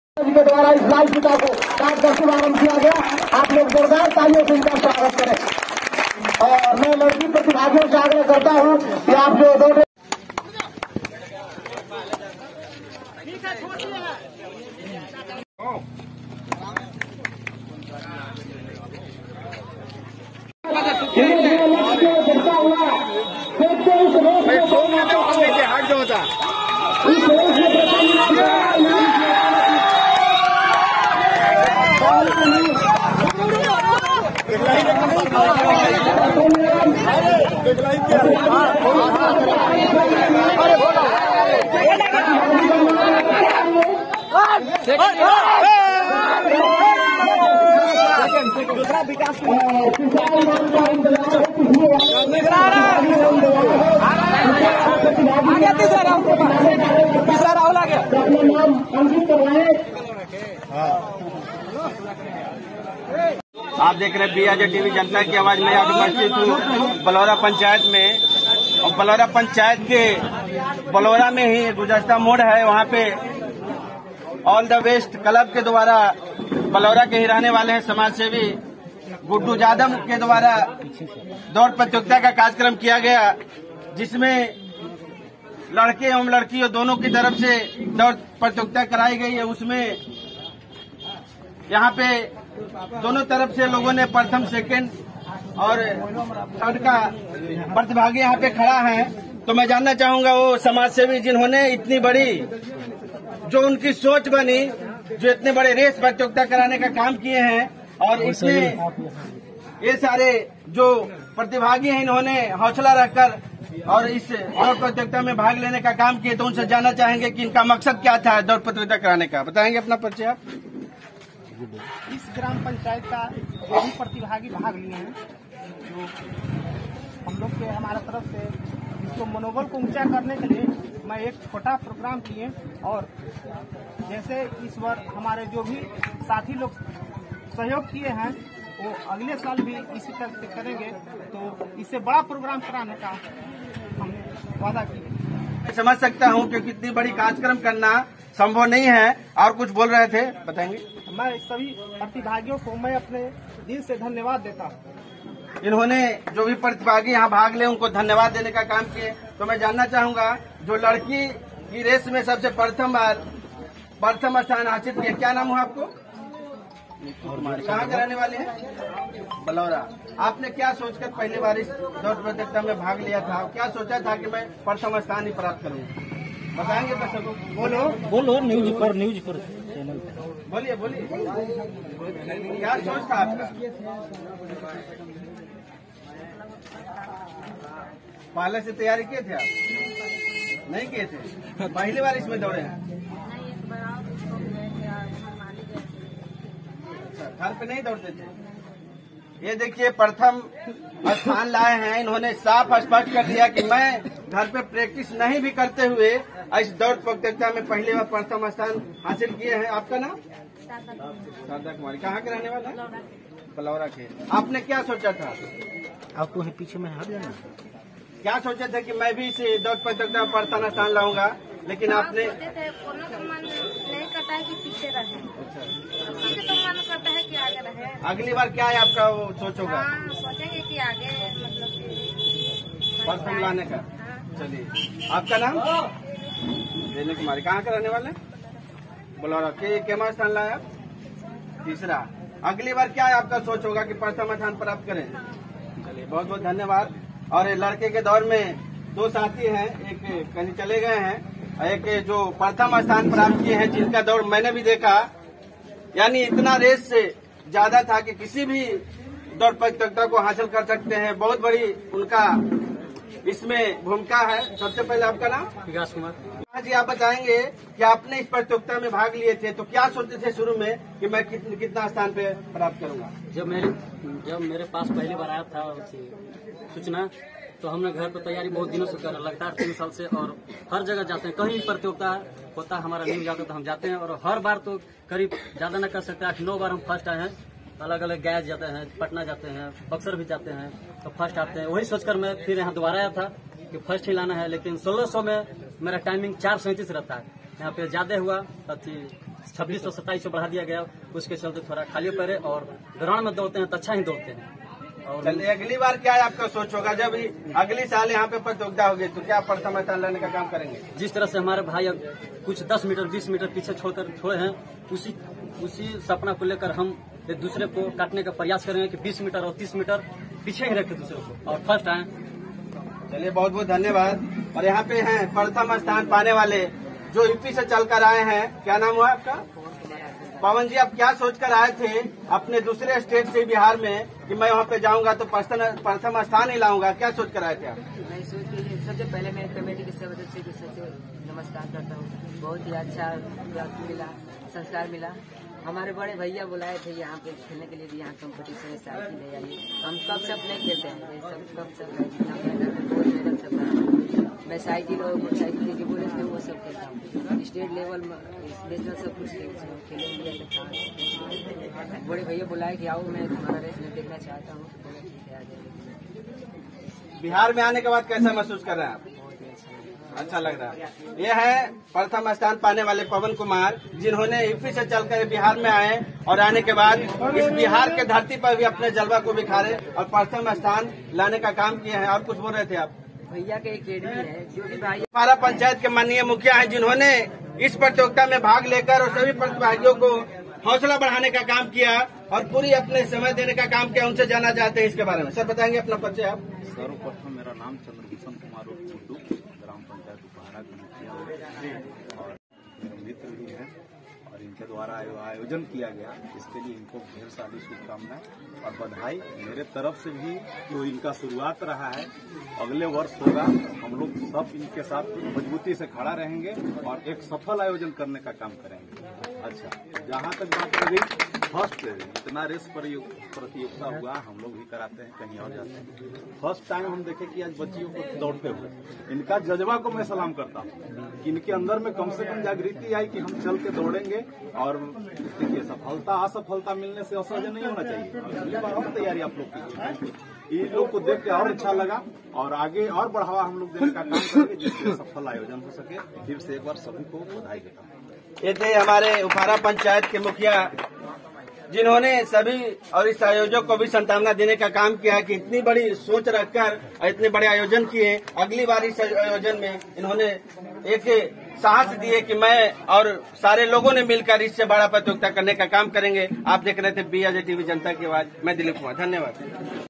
अरवल जिले के ग्राम बलोरा में लड़का एवं लड़कियों का रेस प्रतियोगिता कराया गया।
ऑल द बेस्ट क्लब के द्वारा अरवल जिले के ग्राम बलोरा में लड़का एवं लड़कियों का रेस प्रतियोगिता कराया गया।